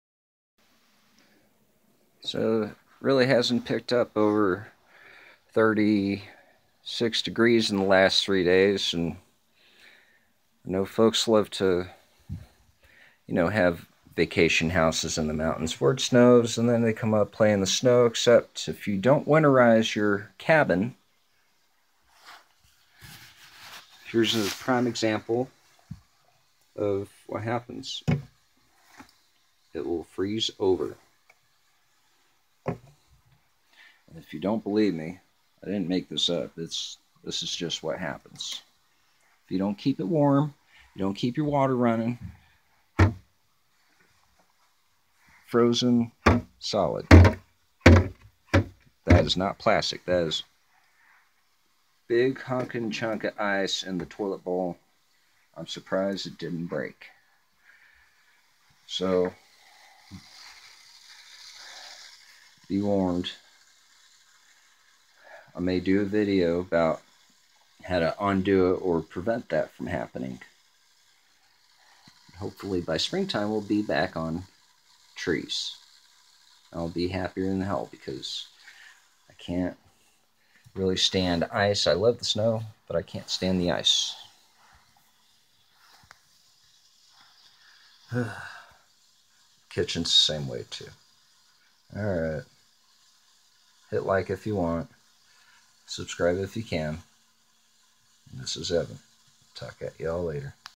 Cabin frozen over
Plumbing issue and toilet ice cube. No nasty surprise, just watch and see what winterizing can prevent.